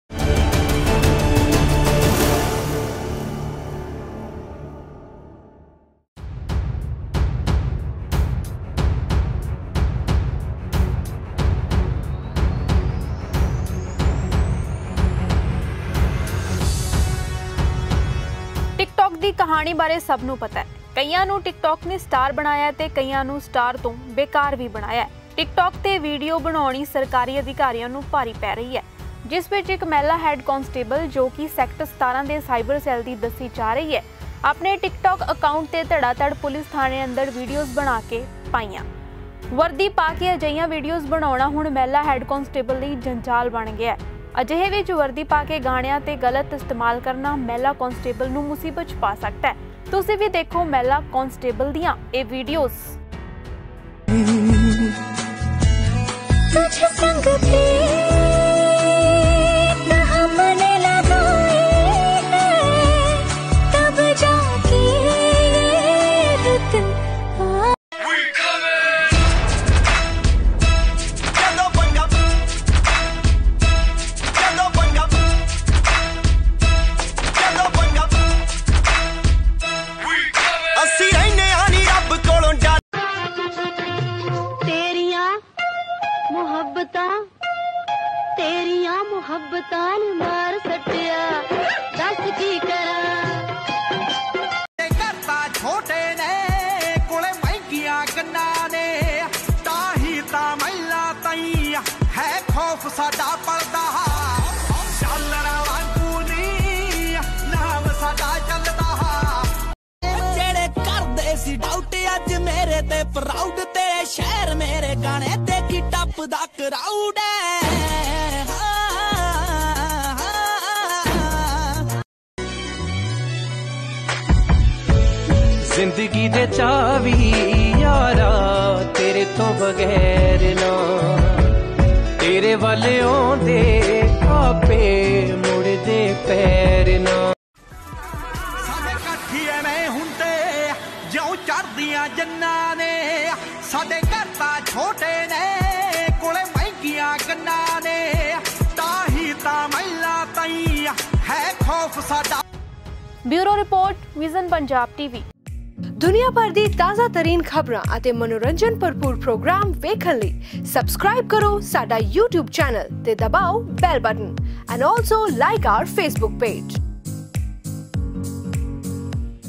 Head Constable Of Cyber Crime Made Controvery on Tik Tok
ਚੰਡੀਗੜ੍ਹ ਦੀ ਮਹਿਲਾ ਹੈਡ ਕਾਂਸਟੇਬਲ ਦਾ ਟਿਕ ਟੋਕ ਕਰੇਜ ਆਇਆ ਸਾਹਮਣੇ
 ਥਾਣੇ 'ਚ ਟਿਕ ਟੋਕ 'ਤੇ ਬਣਾਈਆਂ ਵੀਡੀਓਸ
 ਵਰਦੀ ਪਾ ਕੇ ਥਿਰਕੀ ਗਾਣਿਆ ਦੀ ਤਾਨ 'ਤੇ
 ਹੋ ਸਕਦੀ ਏ ਕਾਰਵਾਈ